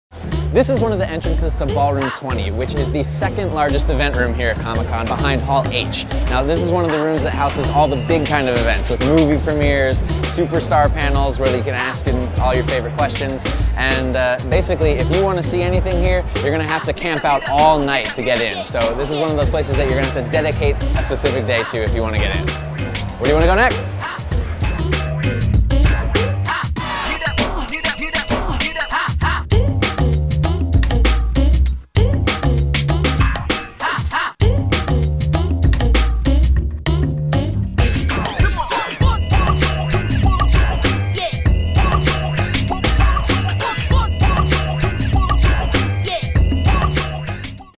Ballroom 20 - Choose Your Own SDCC
The second largest panel room at the Con. Ask your favorite stars questions and learn their secrets.